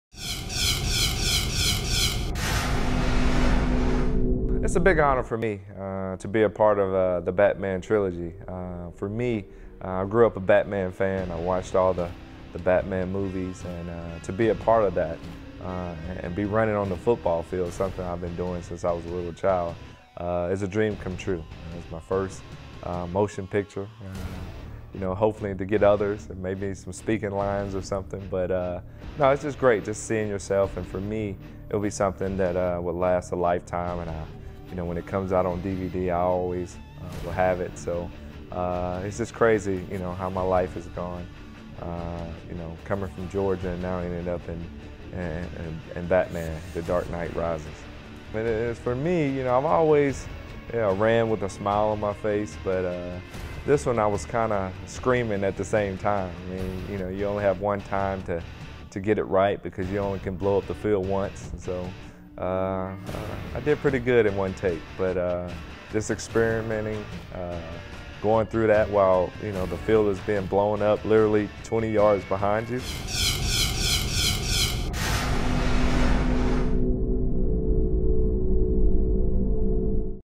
UGA Football: Hines Ward in The Dark Knight Rises: 2012
Watch this exclusive Hines Ward interview as he discusses his role in the new Batman movie. See the rest and more for FREE